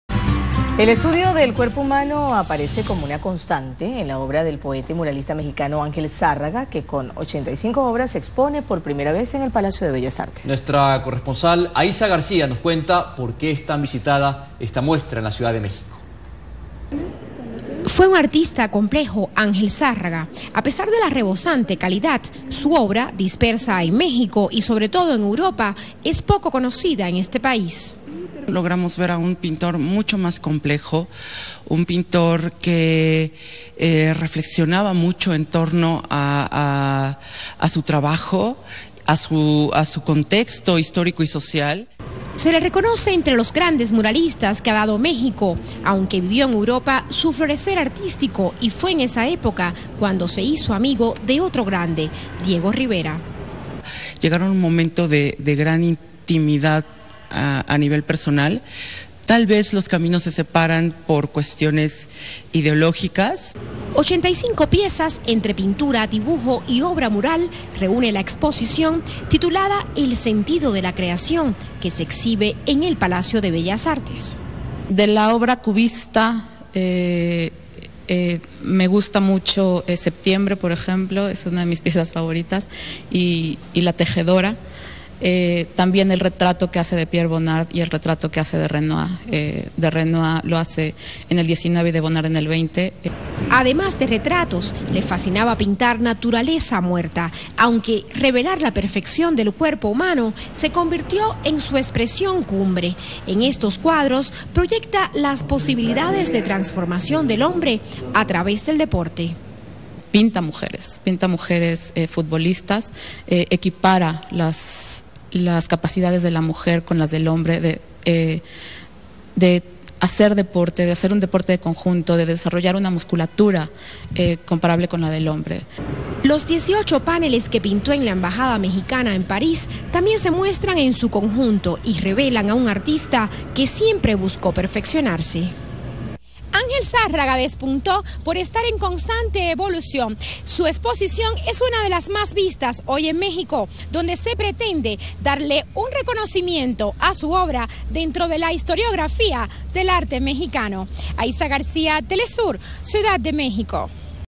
Ángel Zárraga en el Palacio de Bellas Artes de México
27/05/2014

Fue inaugurada una exposición pictórica del pintor mexicano Ángel Zárraga en el Palacio de Bellas Artes. Retratos, murales, cuadros que buscan constantemente reflejar la perfección del cuerpo humano, conforman esta muestra con la que se reconoce el valor de este artista plástico, quien fue amigo de Diego Rivera. teleSUR